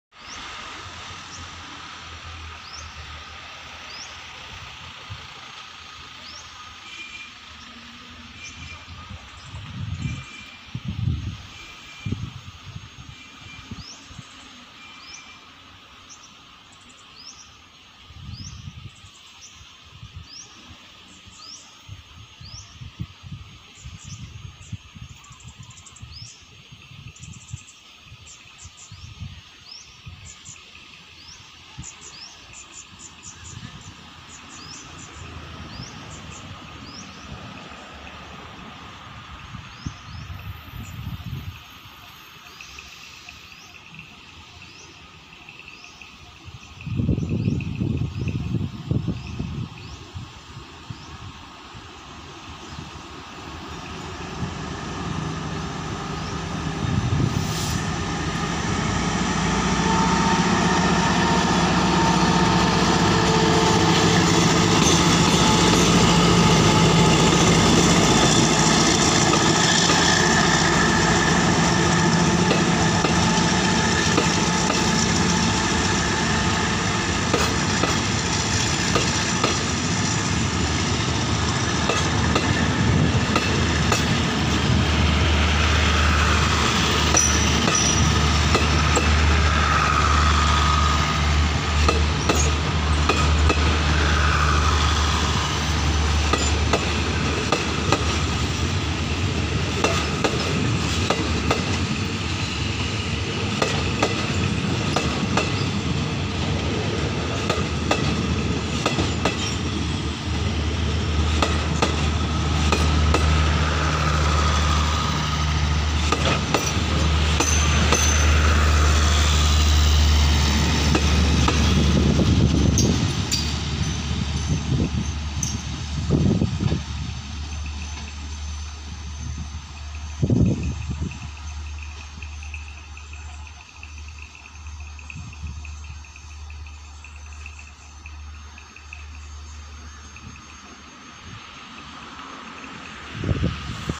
ขบวนรถด่วนพิเศษทักษิณ 38 เข้าสู่สถานี นครปฐม ALSTHOM 4107 CAT
ถ่ายในวันพุธที่ 17 มีนาคม 2564 เวลา 12:05 น.